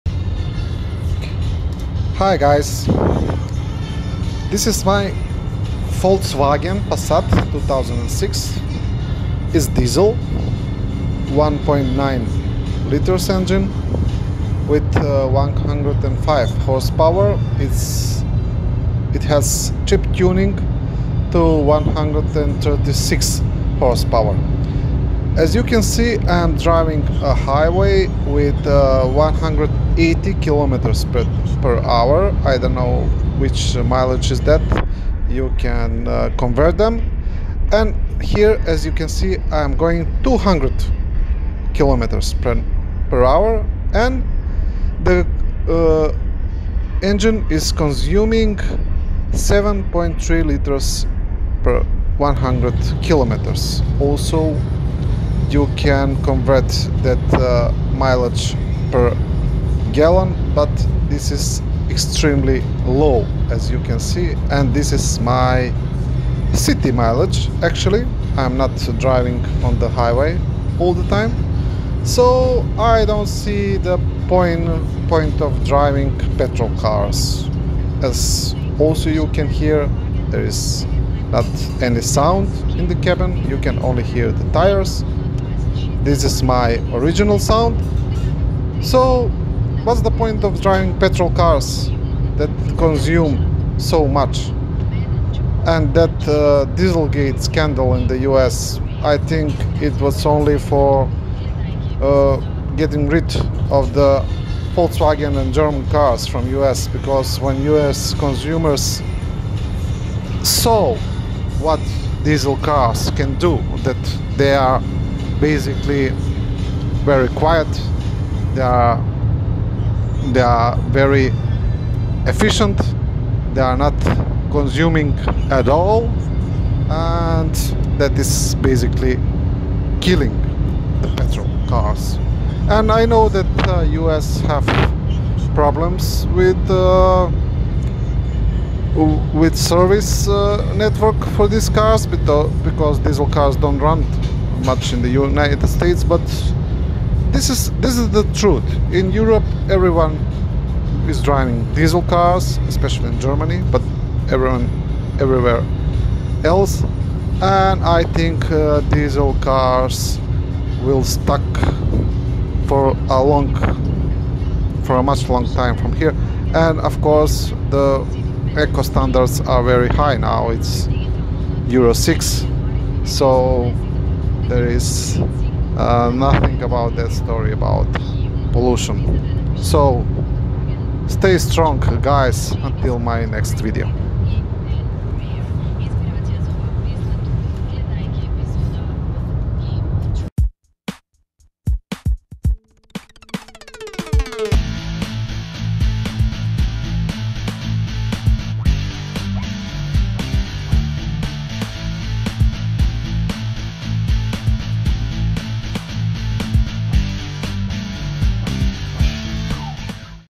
WHY AMERICANS ARE WRONG ABOUT DIESEL CARS, THE CONSPIRACY TO KICK DIESEL OUT OF THE UNITED STATES
This is VW Passat 1.9 TDI from 2006, with 105 HP, with chip tuning up to 136 HP.

I'm Driving (200 km/h) 125 mph, with consuption of 7,3 L/100 km, or 32 MPG

It's Insane to drive any petrol car with this perfomance, and this is basicly the truth